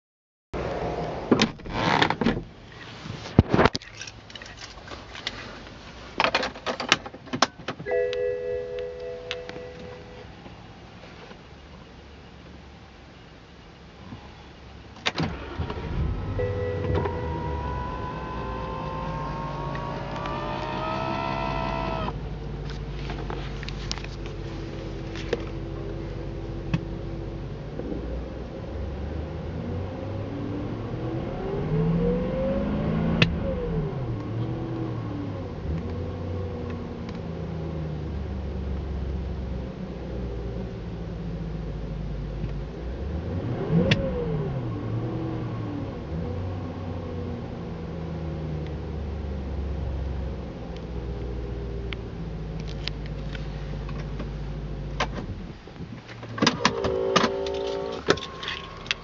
E65 N62B36A Kaltstart und Leerlauf - Drehzahlproblem
Seltsames Problem (?) beim 735i ... normal is das nich, oder?!